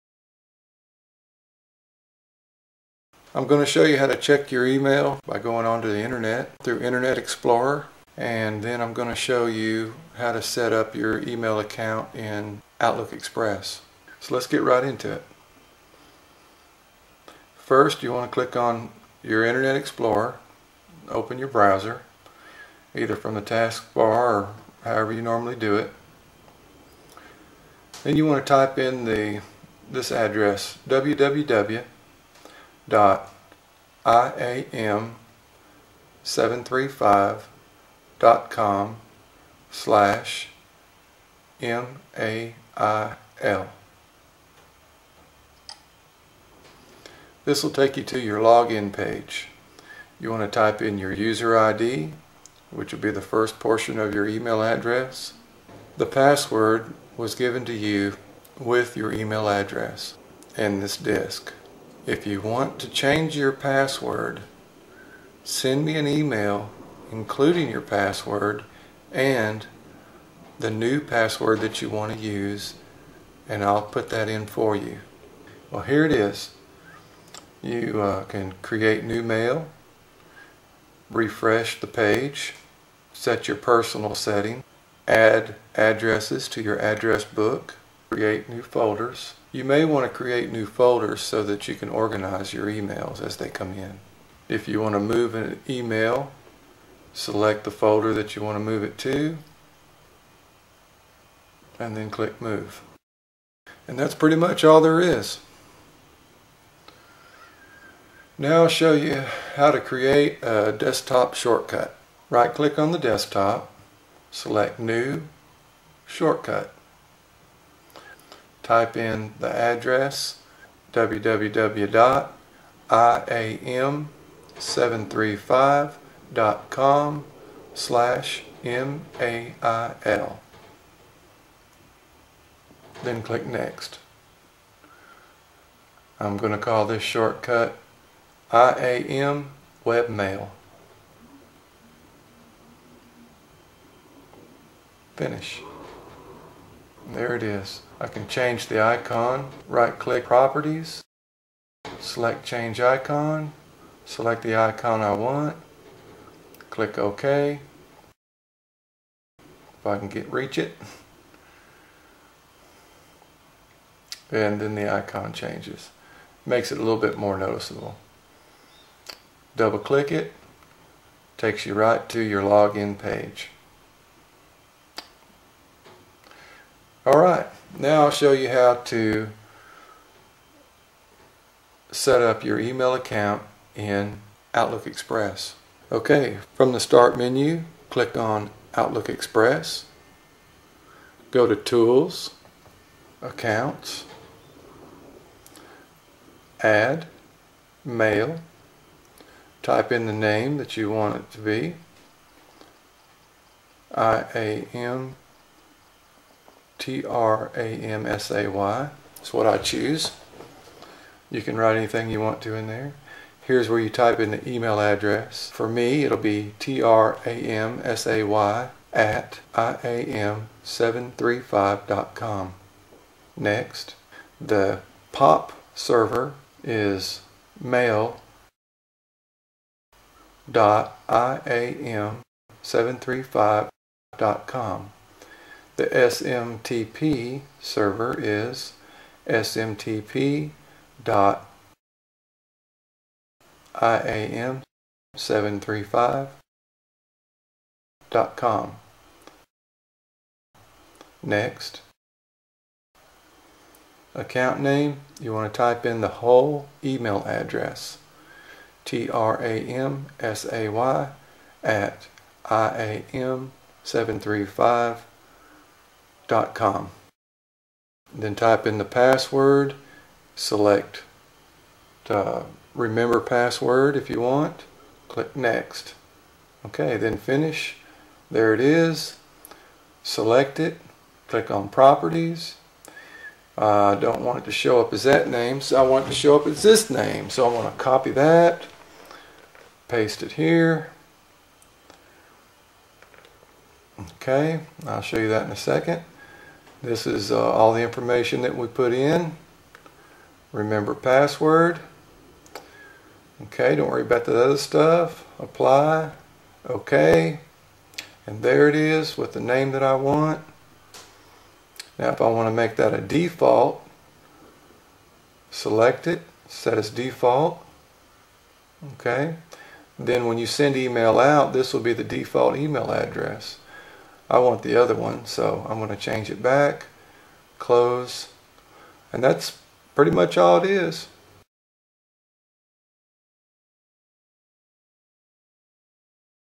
Outlook express email setup
This is how to set up you email account in Outlook Express.